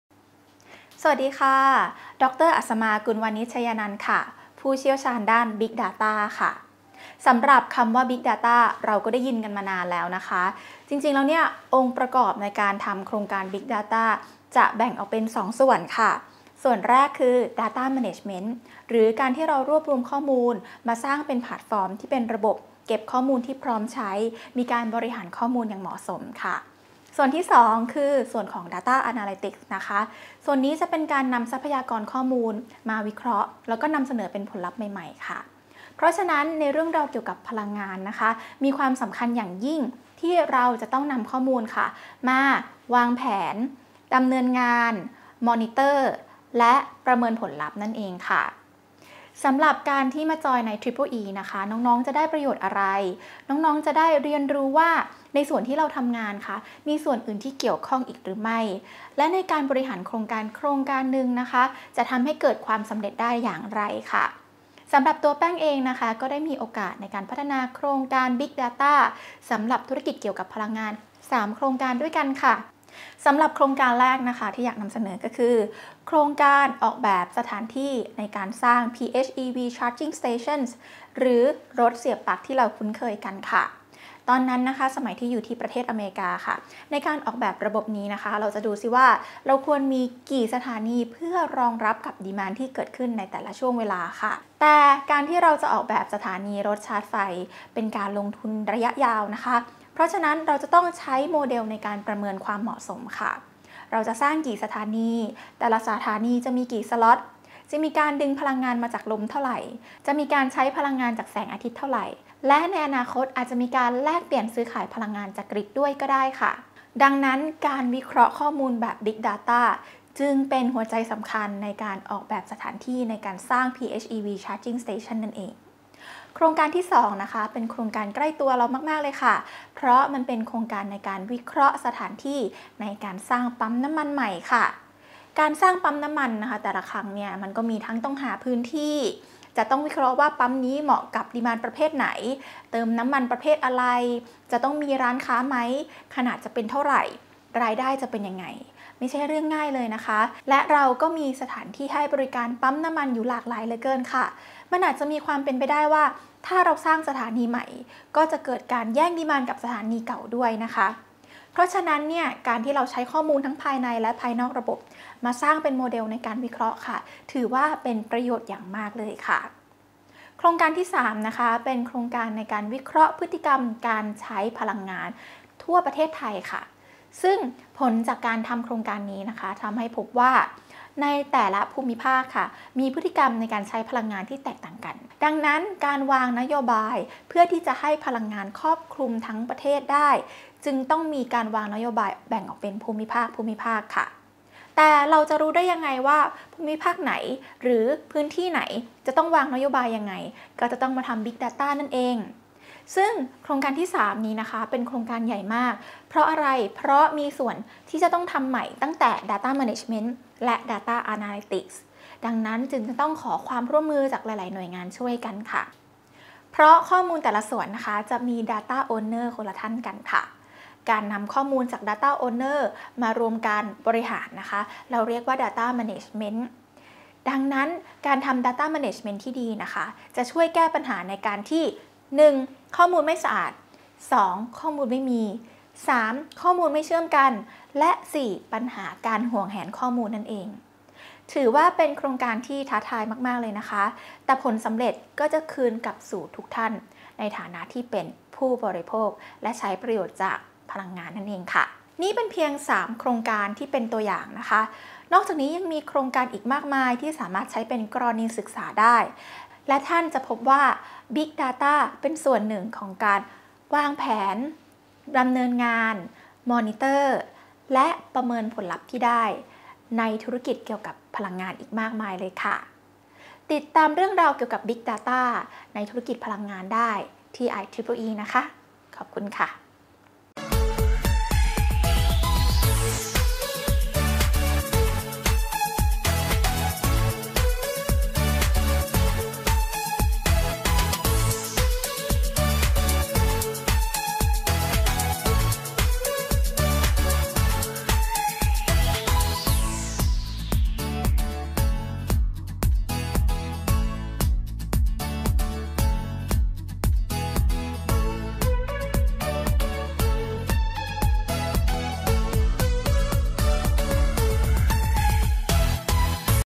สัมภาษณ์ ดร.อสมา กุลวานิชไชยนันท์ CEO & Founder, Coraline Co., Ltd.
สัมภาษณ์ ดร.อสมา กุลวานิชไชยนันท์
CEO & Founder, Coraline Co., Ltd.

Member, IEEE Power & Energy Society - Thailand

งานสัมมนาเชิงวิชาการ  เรื่อง การเปลี่ยนแปลงรูปแบบการจัดการด้านดิจิทัลของอุตสาหกรรมพลังงาน ในวันที่ 8 - 10 มีนาคม 2564 ณ โรงแรม เดอะ สุโกศล กรุงเทพฯ

The Digital Transformation of the Energy Industry:
AI, Machine Learning, IoT, Blockchain, Cloud, Big Data and Cyber Security